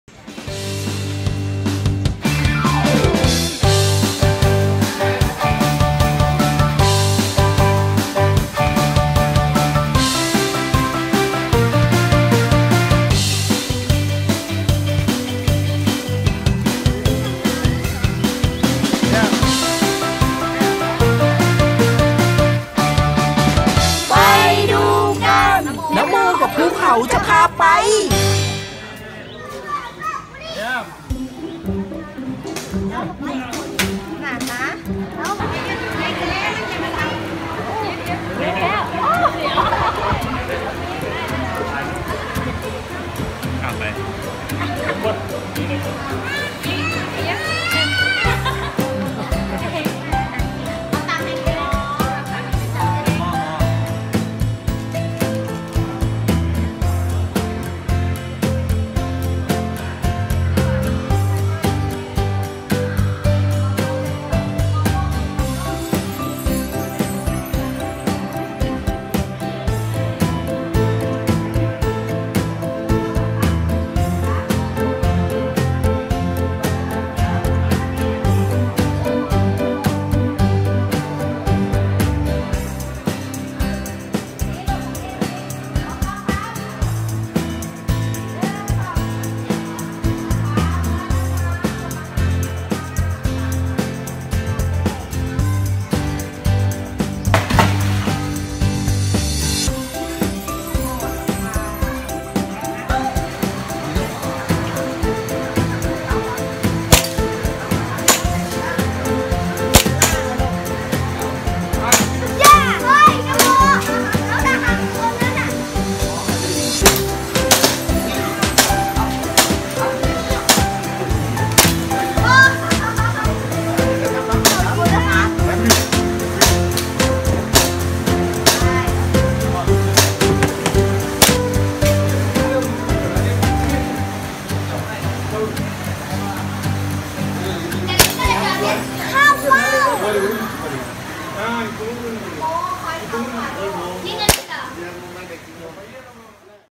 สวนเสือศรีราชา จ ชลบุรี
วันนี้ นะโม ภูเขา และน้องทะเล พาไปเที่ยวสวนเสือศรีราชา จ.ชลบุรี